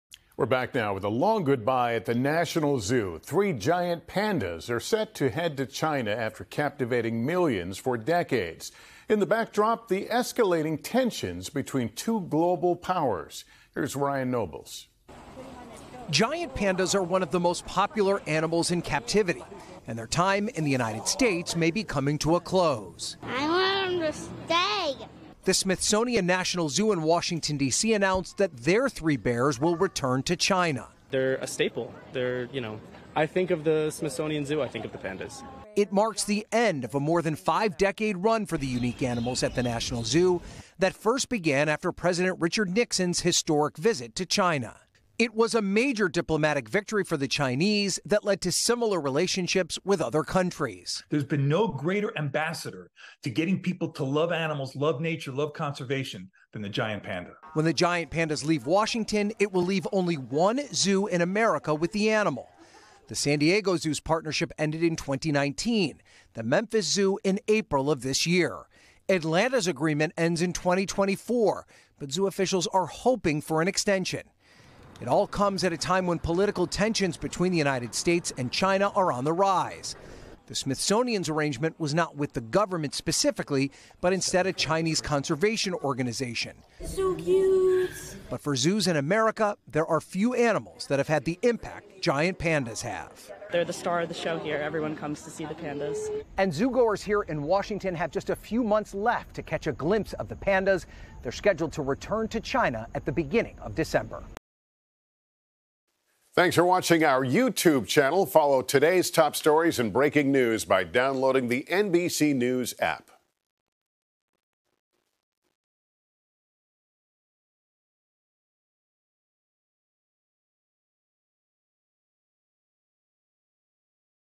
Pandas set to leave National Zoo and return to China after more than five decades
The Smithsonian National Zoo in Washington, D.C. announced that their three giant pandas will return to China after more than five decades. NBC News’ Ryan Nobles has more details on the end of the bears’ historic time in the U.S.